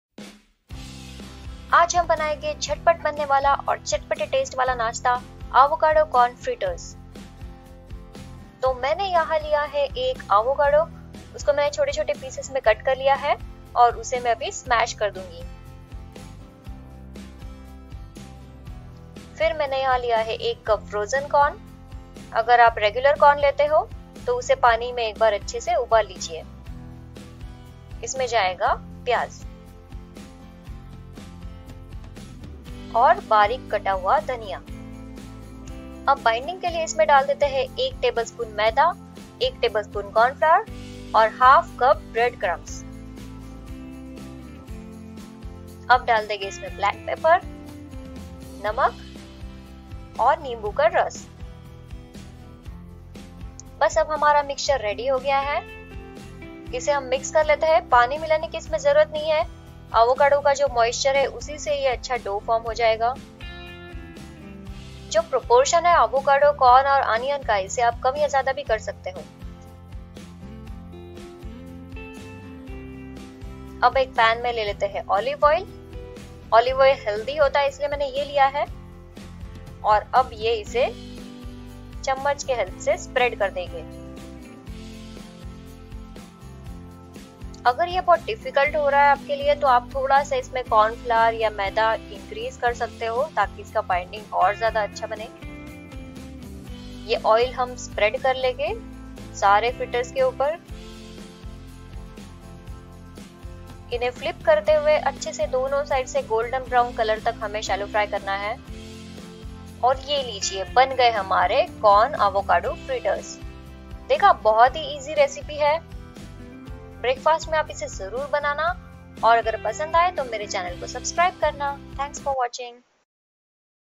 Try this recipe once and you will make it again | Avocado Corn Fritters | Healthy Breakfast Recipe
Corn Avocado Fritters | Corn Avocado Tikki | Corn ka healthy breakfast

Try it out and enjoy ! Do share your experiences through comments.